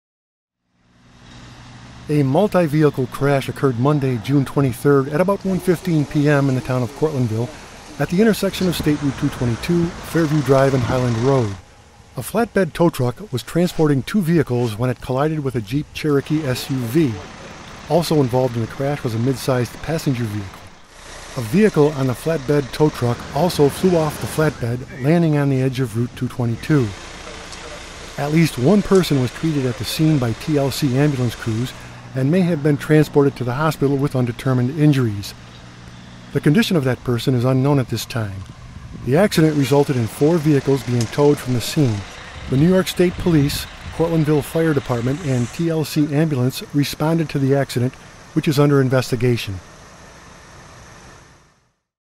Tow Truck Involved in Multi Vehicle Crash
A multi vehicle crash occurred Monday June 23rd at about 1:15 pm in the town of Cortlandville at the intersection of State Route 222, Fairview Drive and Highland Road.  A flat bed Tow Truck was transporting two vehicles when it collided with an Jeep Cherokee SUV. Also involved in the crash was a mid sized passenger vehicle. A vehicle on the flat bed Tow Truck also flew off the flatbed, landing on the edge of Route 222. At least one person was treated at the scene by TLC Ambulance and may have been transported to the hospital with undetermined injuries.  The condition of that person is unknown at this time. The accident resulted in four vehicles being towed from the scene. The New York State Police, Cortlandville Fire Dept. and TLC Ambulance responded to the accident, which is under investigation.